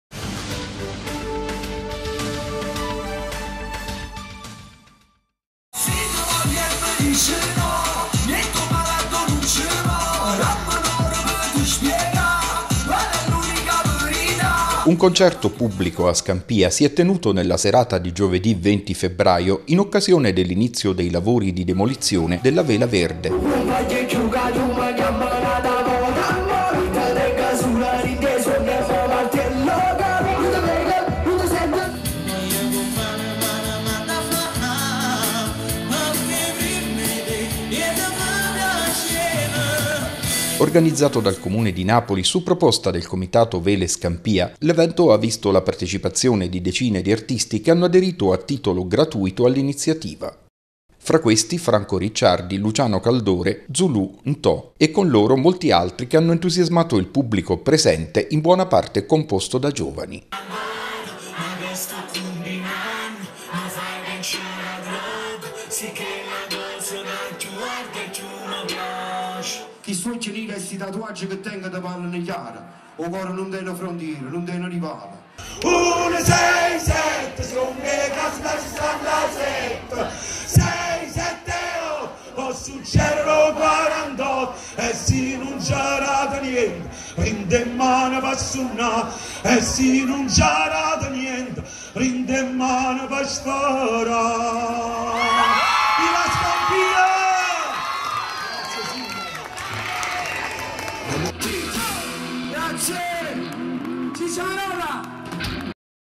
Scampia canta. Concerto per la rottamazione della vela verde
Un concerto pubblico a Scampia con artisti del territorio in occasione del primo giorno di lavori per l'abbattimento della vela verde.